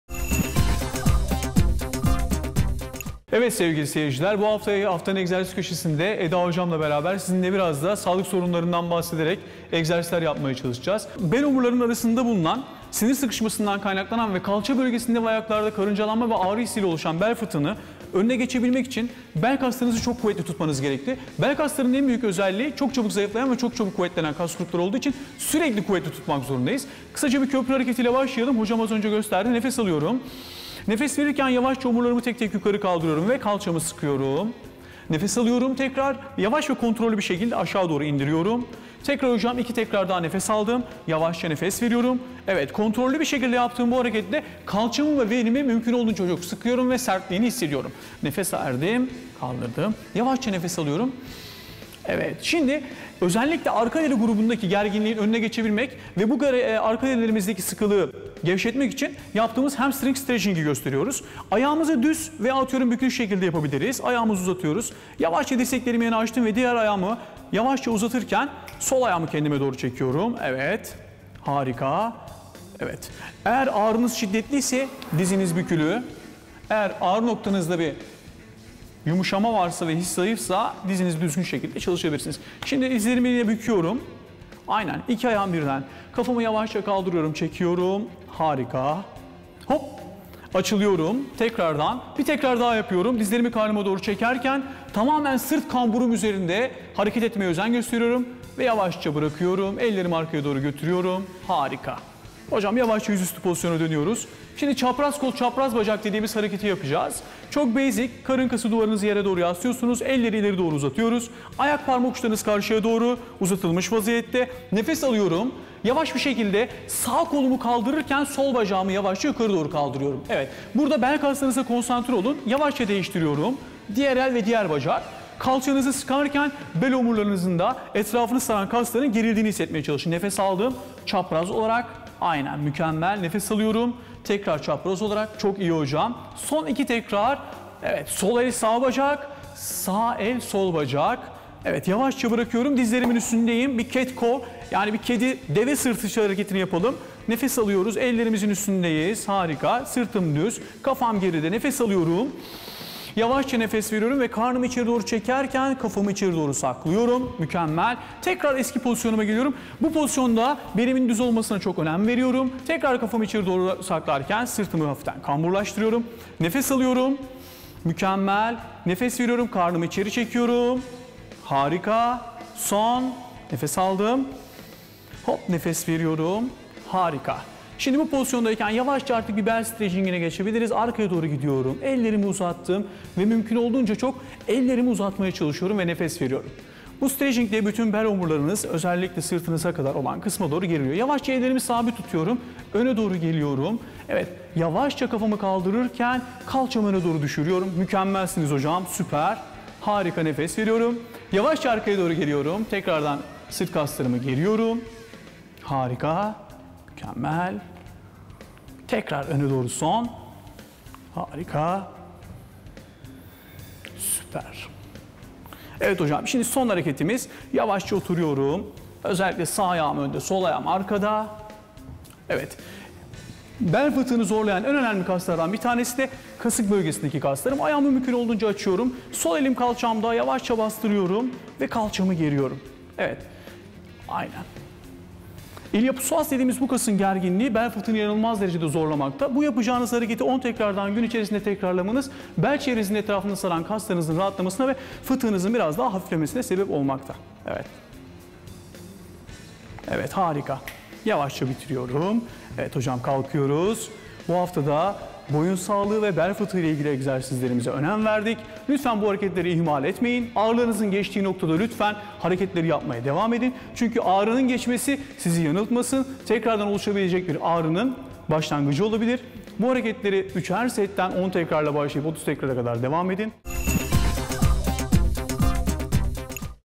Bel bölgemizi nasıl güçlendirebiliriz?
Bel ağrılarınızı azaltabilecek ve fit görünmenizi sağlayabilecek bel egzersizlerini videomuzdan izleyebilirsiniz.